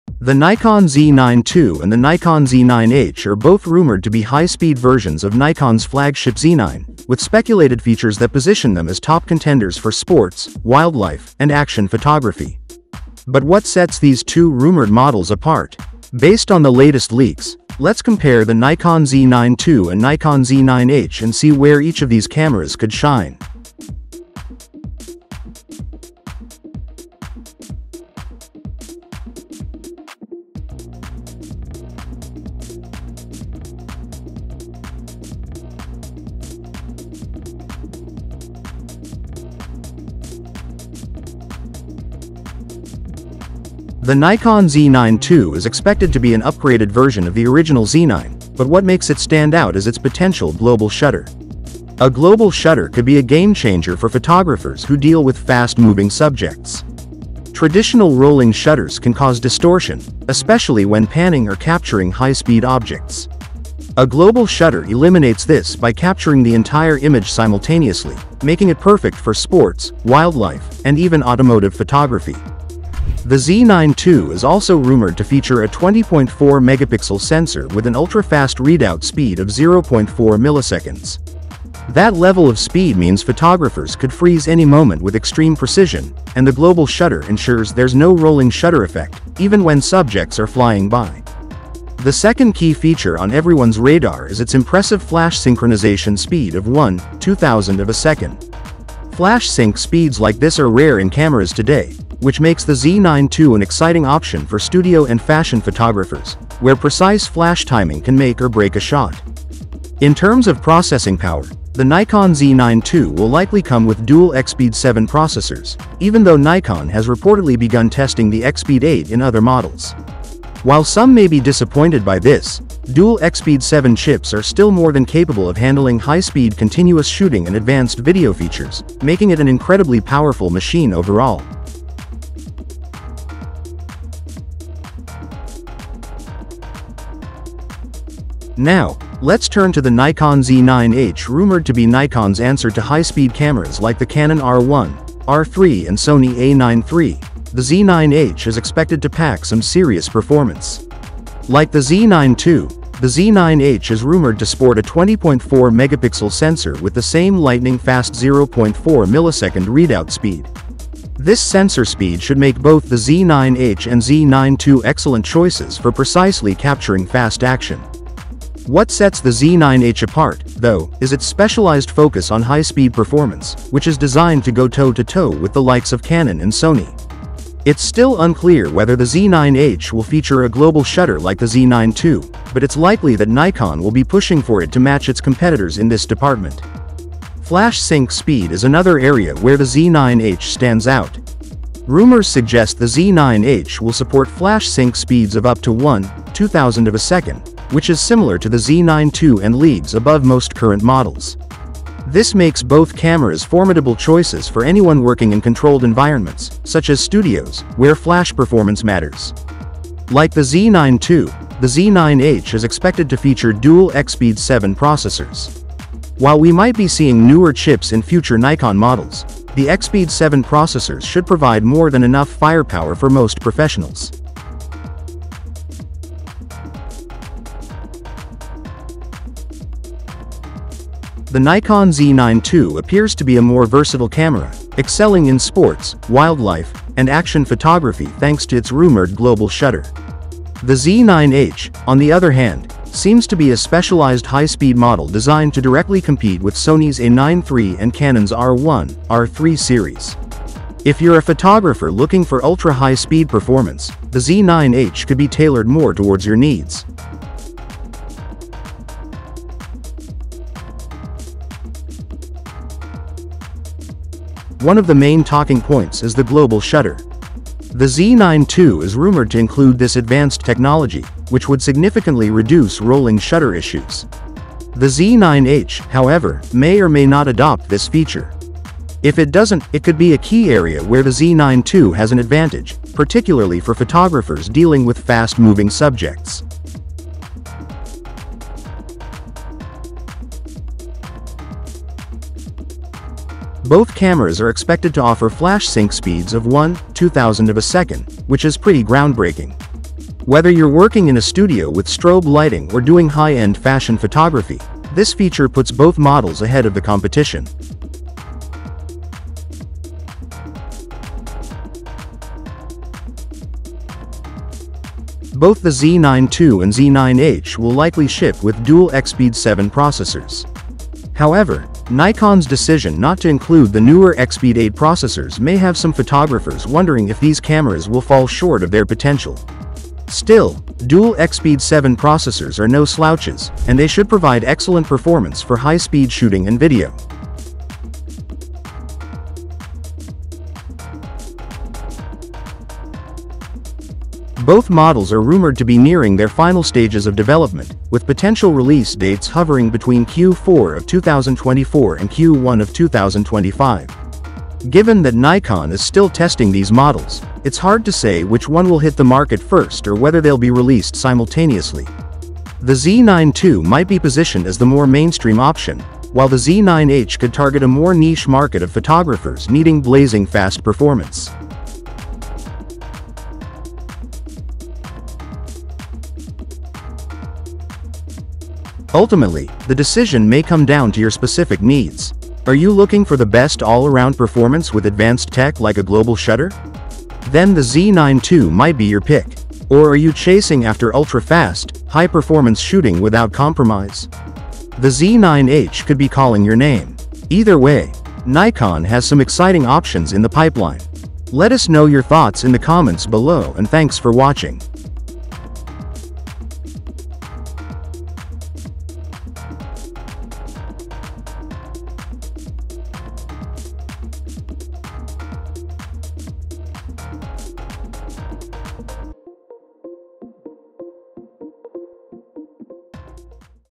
Nikon Z9 II vs Nikon Z9H - Nikon's Future Plan?
Nikon Z9 II and the Nikon Z9H are both rumored to be high-speed versions of Nikon’s flagship Z9, with speculated features that position them as top contenders for sports, wildlife, and action photography. But what sets these two rumored models apart? Based on the latest leaks, let's compare the Nikon Z9 II and Nikon Z9H and see where each of these cameras could shine.
00:00 - Intro
00:44 - Nikon Z9 II
02:19 - Nikon Z9H
03:51 - Target Audience
04:25 - Shutter Type
04:54 - Flash Sync Speed
05:16 - Processing Power
05:45 - Release Timeline and Speculation
06:19 - Final Thoughts